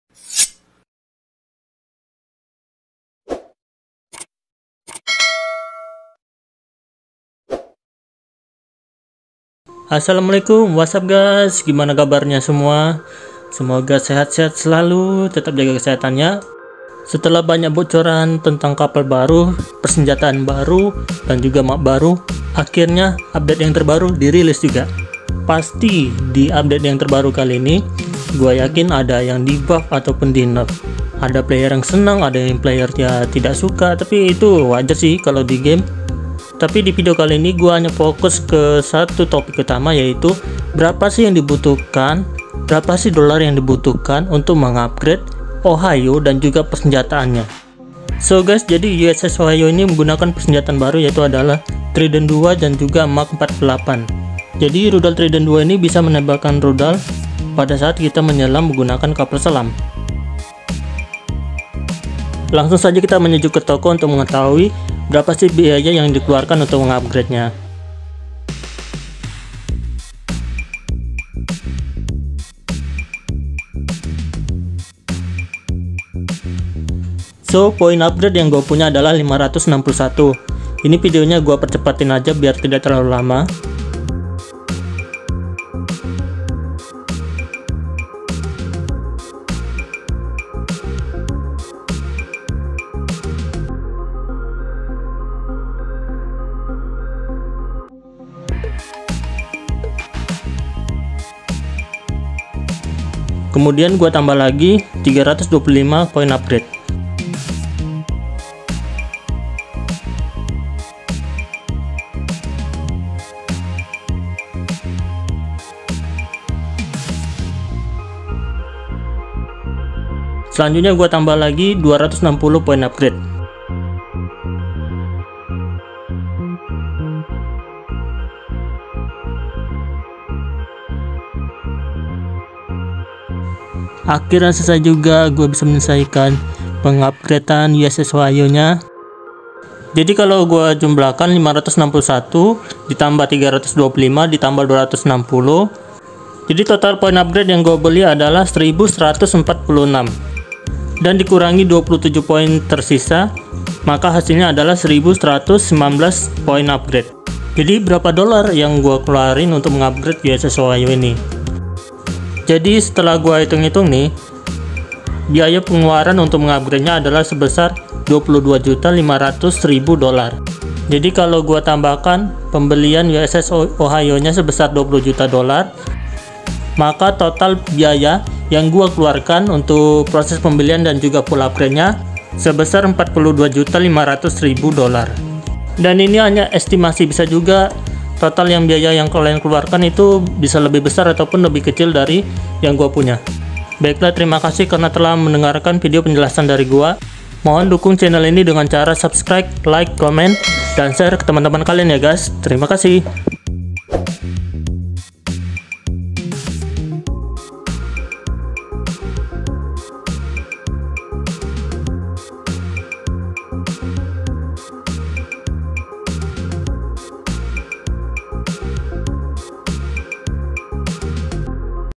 Biaya Full Upgrade USS OHIO
Hy guys..

Di video ini gua akan berbagi info seputar biaya yang harus dikeluarkan untuk upgrade USS OHIO.

Silahkan simak videonya ya...